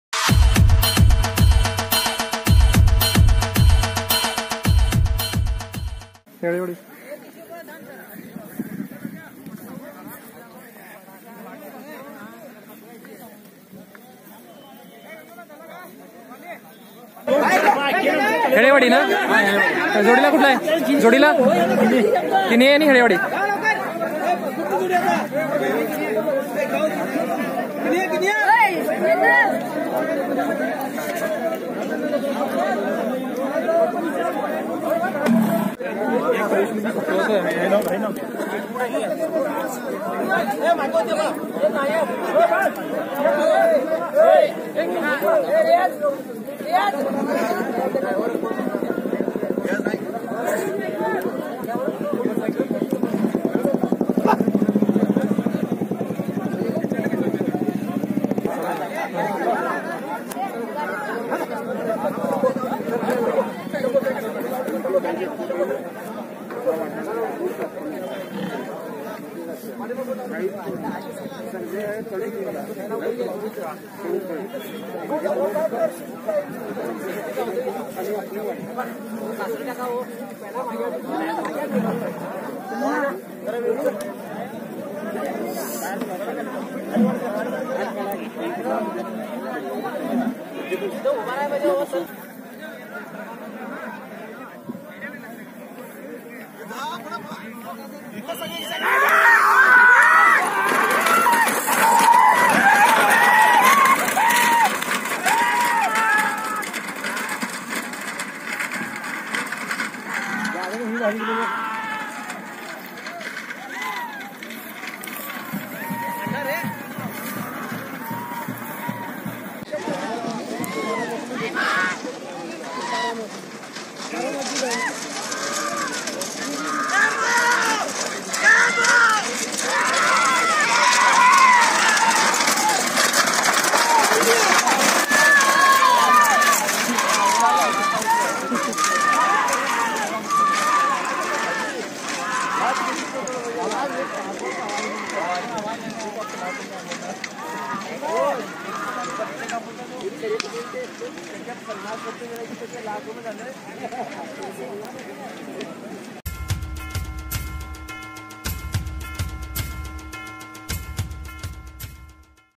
|| Jabardast Khillari Racing Bulls Running in Bullock cart race || Distance Covered : 1585 Feet ||
In this video you can watch khillari racing bulls of Kinaye and Helewadi running in bailgadi sharyat at kuppatgiri khanapur.